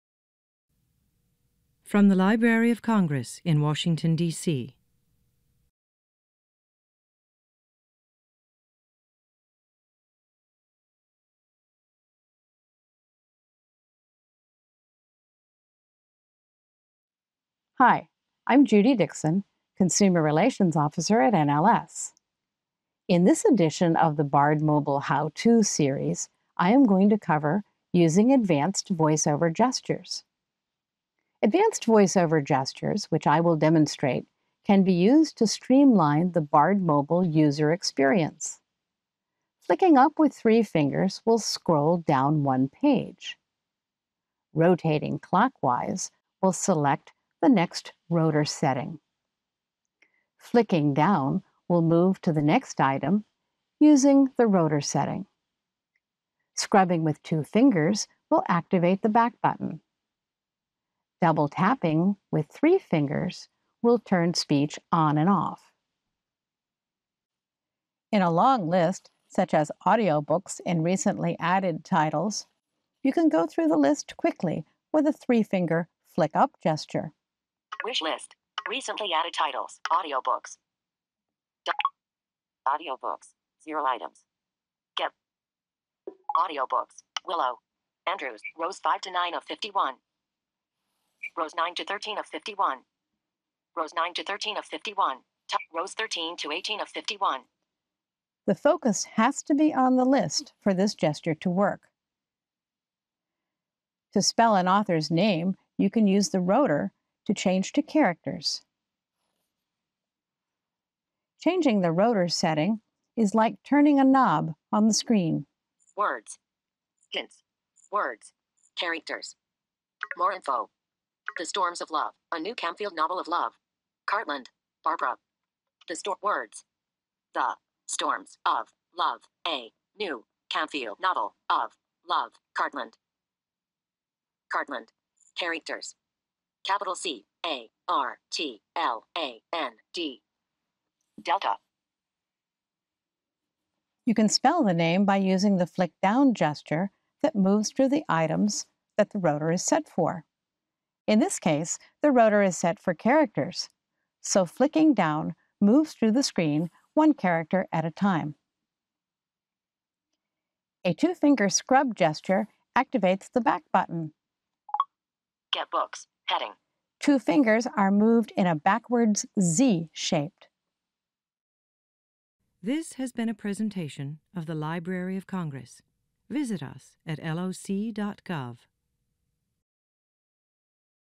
BARD Mobile: Advanced Voiceover Gestures
Judy Dixon provides a tutorial on how to use advanced voiceover gestures with the BARD Mobile app for iOS devices.

Speaker Biography: Judy Dixon is consumer relations officer for the National Library Service for the Blind and Print Disabled (formerly National Library Service for the Blind and Physically Handicapped).